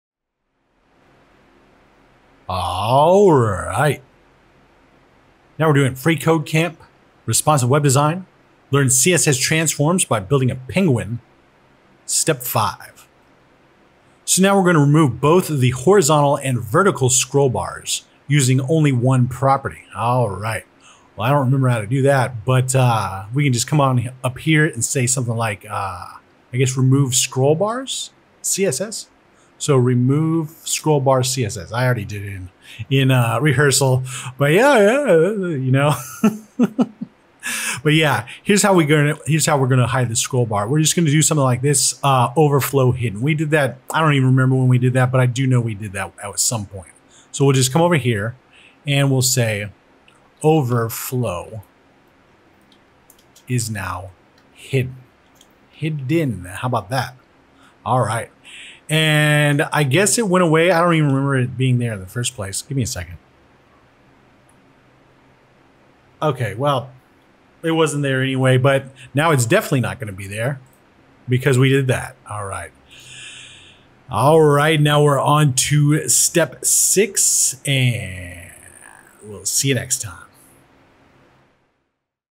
Build a Penguin - Step 5 | Learn CSS & HTML | FreeCodeCamp
🎉 Welcome to Episode 5 of "Flappy Penguin: A CSS Adventure!" - "Smooth Sailing: Removing Scrollbars" 💻

Today's exciting step is all about creating an immersive and distraction-free experience by removing both the horizontal and vertical scrollbars using only one property! 🚫📜

In web development, less is often more. Those pesky scrollbars can divert your user's attention and take away from your stunning design. In this episode, we'll guide you through:

A clean and simplified method to remove scrollbars 🛠️
Ensuring that the user's attention stays on your amazing Flappy Penguin🎨
Crafting a seamless and professional-looking web page 🌐

The absence of scrollbars enhances the aesthetic appeal of the page, leading to a visually pleasing and more engaging user experience. Whether you're a beginner or a pro, this step is essential in honing your web design finesse! 🎨

Join our journey as we perfect our digital canvas and build together a web experience that keeps viewers captivated! ✨